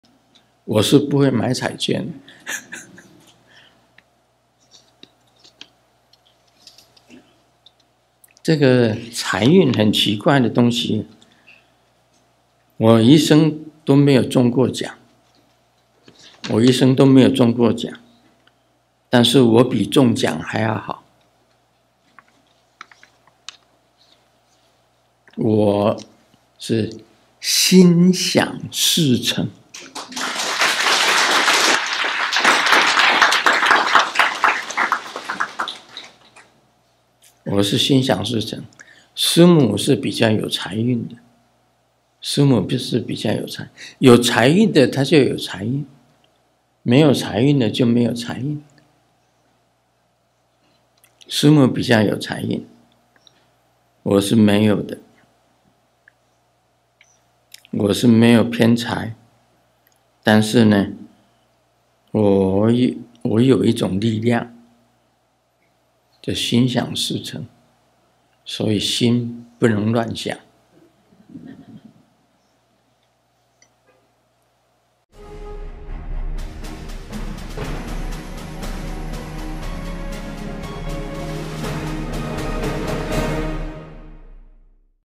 20240128聖尊蓮生活佛盧勝彥開示：師尊沒有中過獎，但都會心想事成！－TBSN高清字幕版
20240128聖尊蓮生活佛盧勝彥開示：師尊沒有中過獎，但都會心想事成！－TBSN高清字幕版